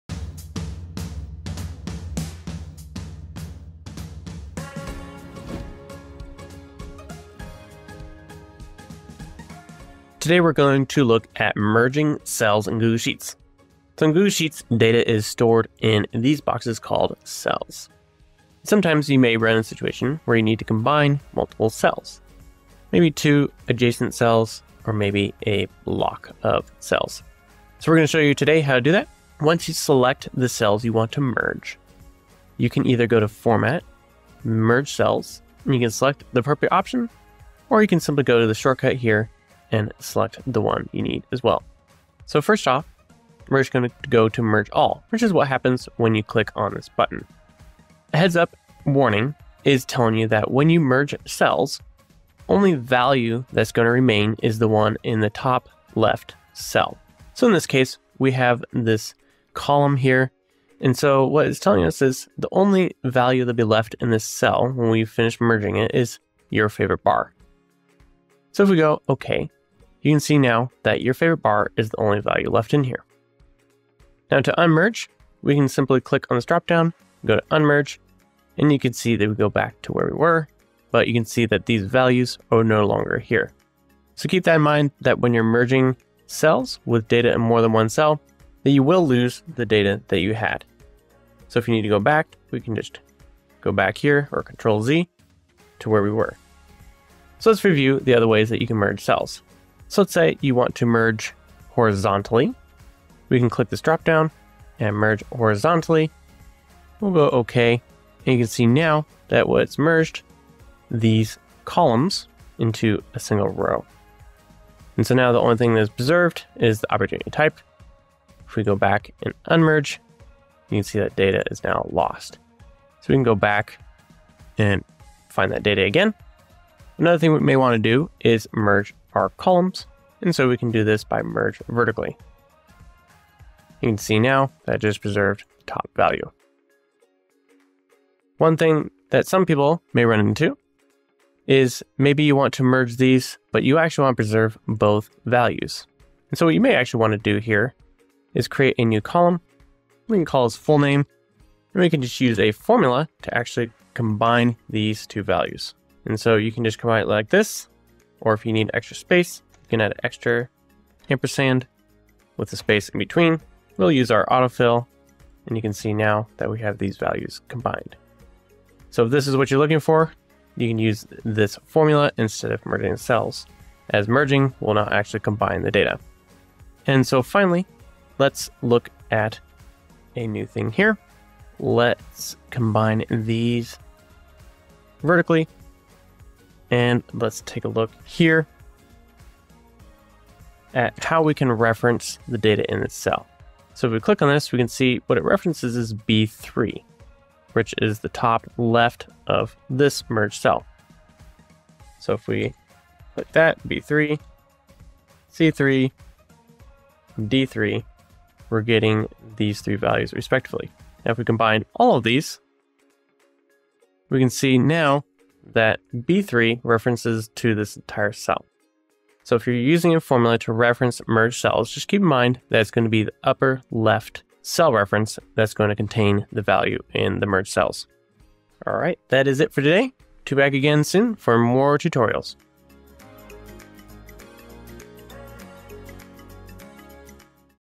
How to Merge Cells in Google Sheets: Combine Horizontally, Vertically, and More
Sometimes you may need to combine multiple cells such as two adjacent cells or a block of cells. Let's take a look at merging cells in Google Sheets.

We'll walk through some examples and talk through the merge cells warning, what to keep in mind along the way, and how to preserve all of the values by combining instead of merging data.

Video Timeline
00:00: Merging Cells in Google Sheets
00:43: Merge All Cells in Google Sheets
01:00: Losing Data While Merging Cells
01:38: Merge Cells Horizontally
02:08: Merge Columns by Merging Cells Vertically
02:18: How to Merge Cells and Preserve Data (Combine Data)
02:57: How to Reference Merged Cell Data